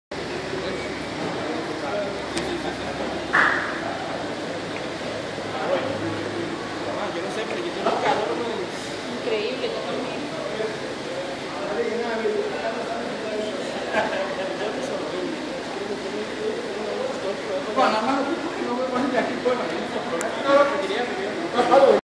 Radio Monumental: Jugadores hondureños en el hotel de concentración
Costa Rica vrs Honduras